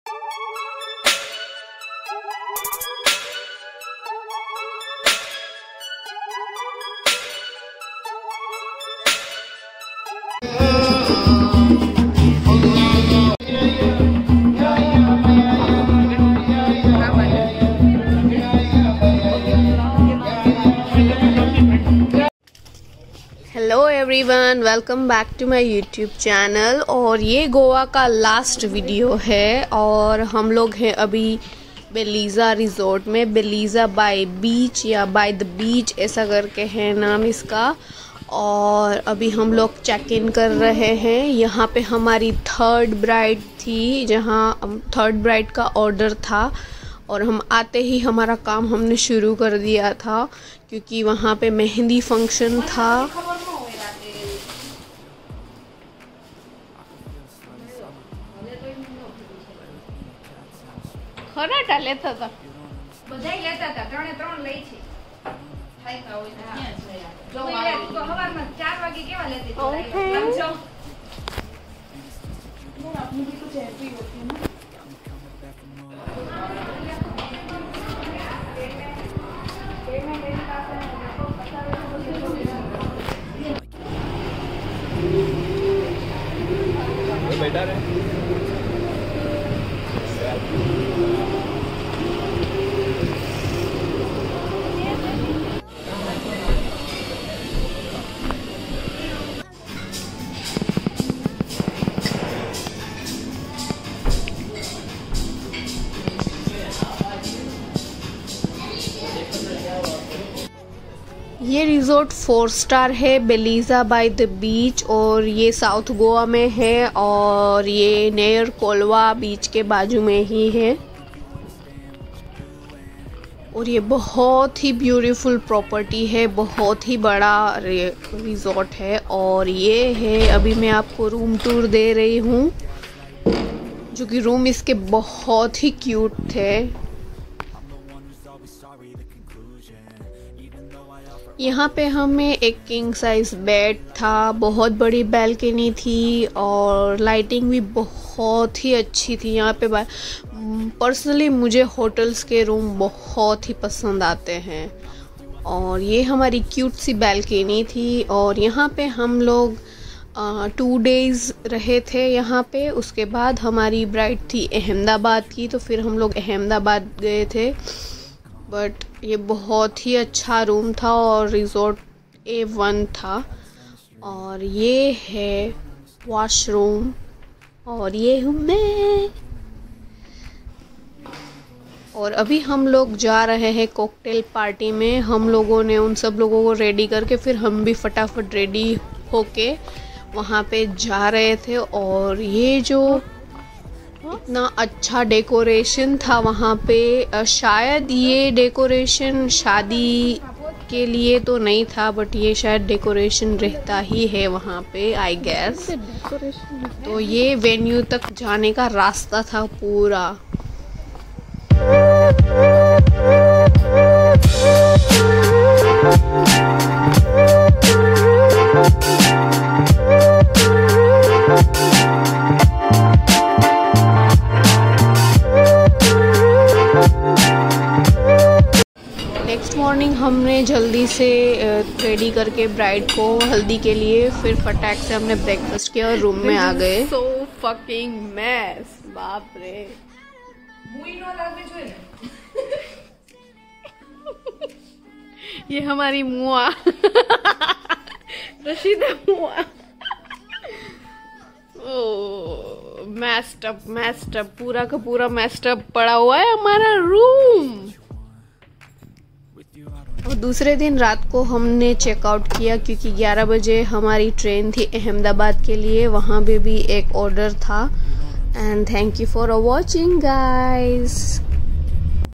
Beleza by the beach GOA | Best Resort in South GOA | GOA Vlog | GOA EP07 | Anju Ahir
Beleza by the beach GOA | Beleza by the beach Resort GOA

In this video I have taken a tour ofBeleza by the beach Resort and have shown some glimpse of the wedding ceremony. Actually I went to this hotel as a makeup assistant for a wedding.

Beleza by the beach Resort is a four star resort located in Betalbatim, South Goa. Beleza by the beach resort is in close to Colva beach. And I think this is the best resort in South GOA.

Address : Colva Beach, Thondwaddo, Betalbatim, Goa

If you have any queries please write in below comment box.